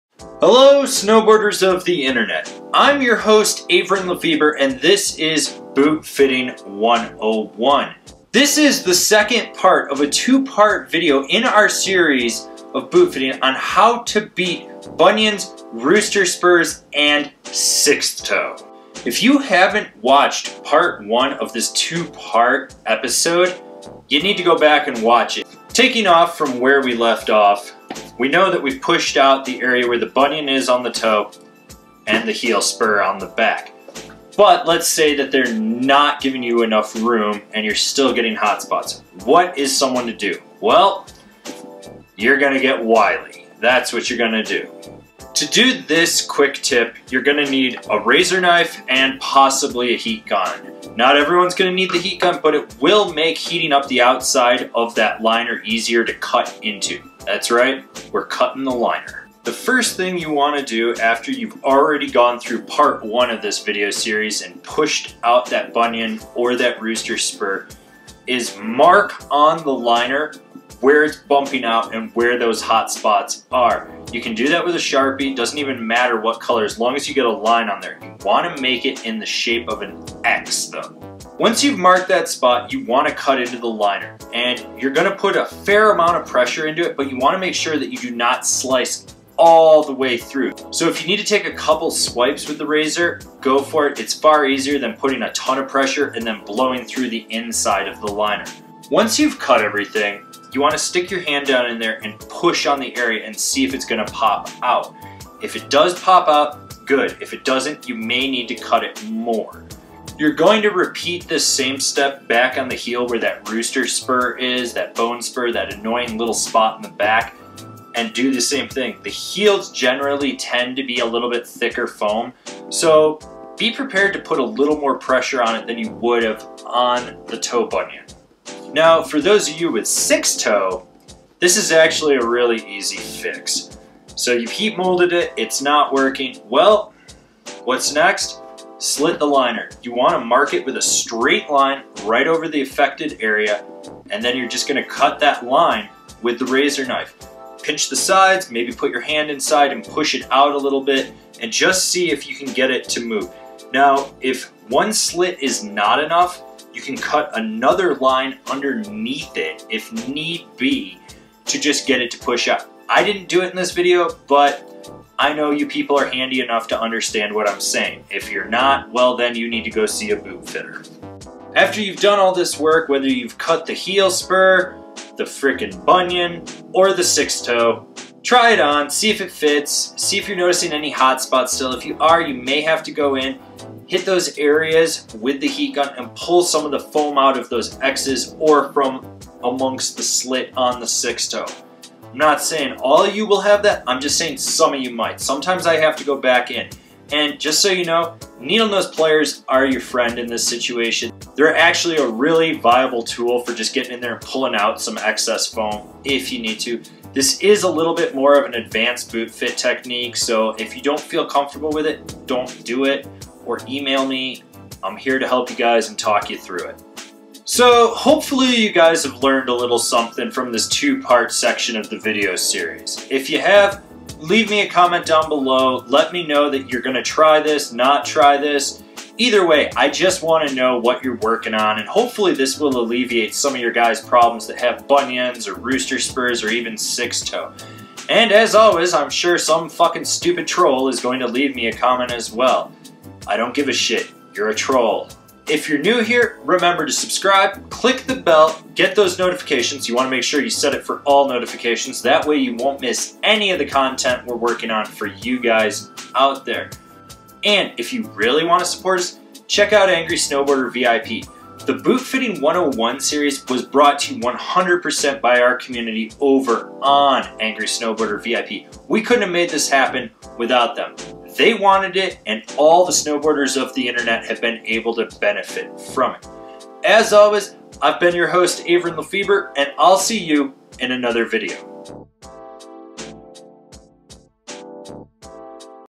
Boot Fitting 101: Beating Bunions and Bone Spurs Pt. 2
July is boot fit month. As such we're going over the basics of how things work and why things happen before we delve into the steps to fix any issues that arise. In this video we're going to go through the second part of combating bunions, bone spurs, rooster spurs, and sixth toe. If this doesn't work, tune into part two.

Hosted, filmed, and edited by Avran LeFeber

This video made possible by the fine folks of Angrysnowboarder.vip